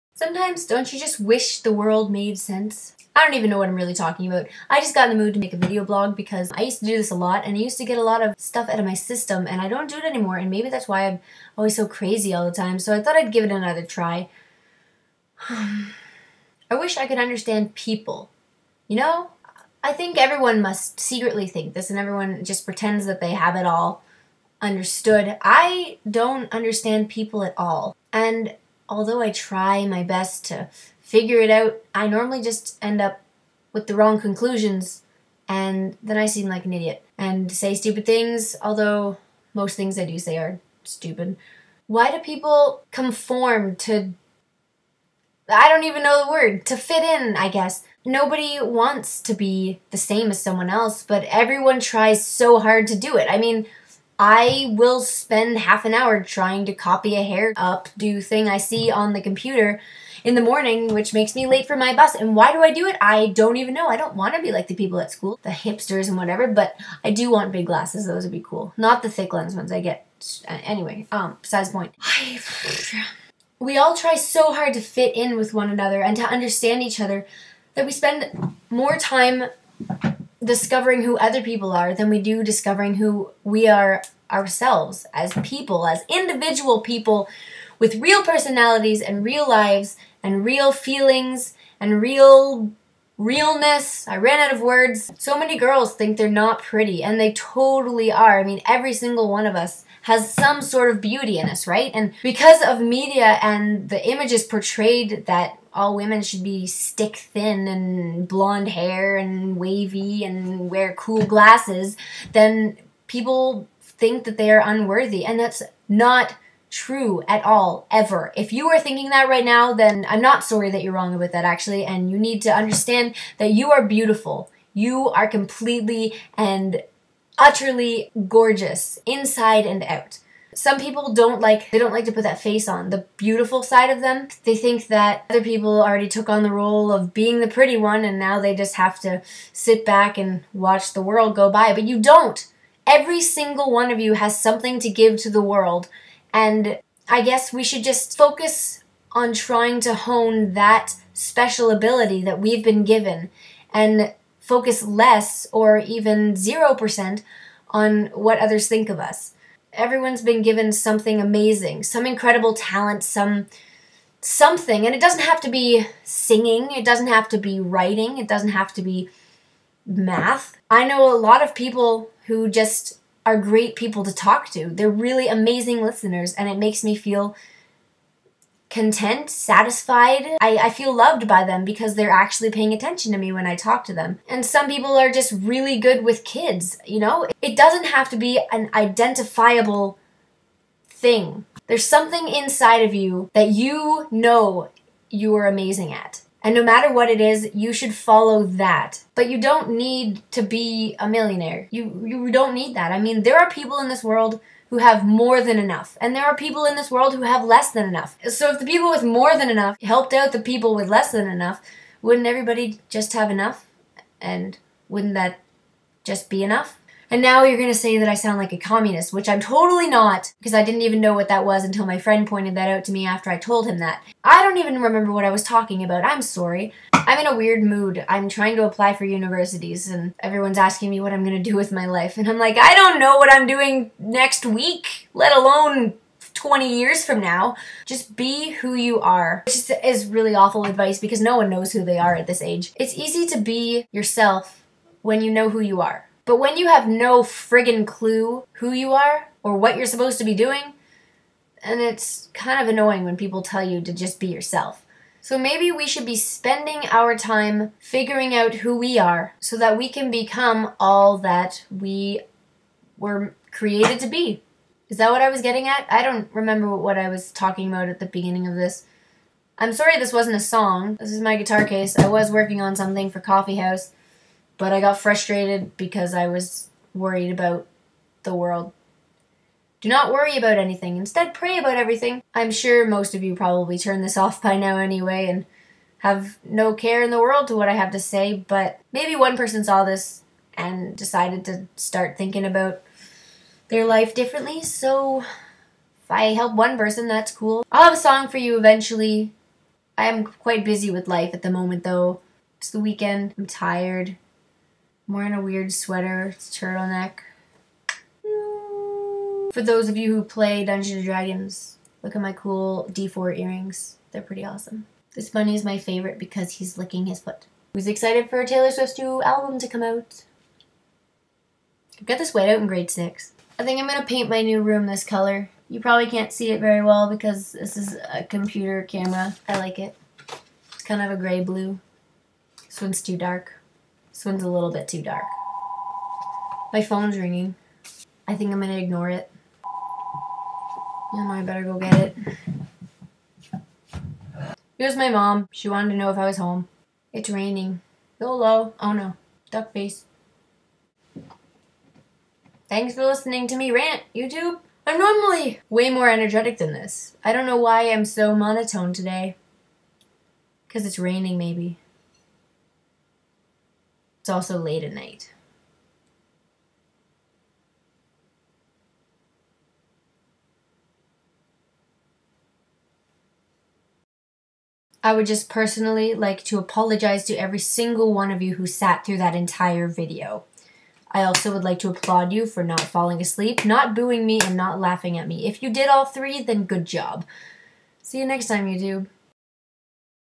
The Condemnation of Comformity
Why are we all striving to be someone else?

A NOTE TO BOYS: I know I only reallytalked about girl issues specifically here, but know that you are beautiful too. Every single of of you. and handsome. And just all around stellar.

Everybody's just great. And I love you all. REALLY.
And so does God. :)

YOLO!!1!?

Duck face, what?

I'm going to eat grapes and watch the special features DVD that came with my box set of Heartland Season 4 videos.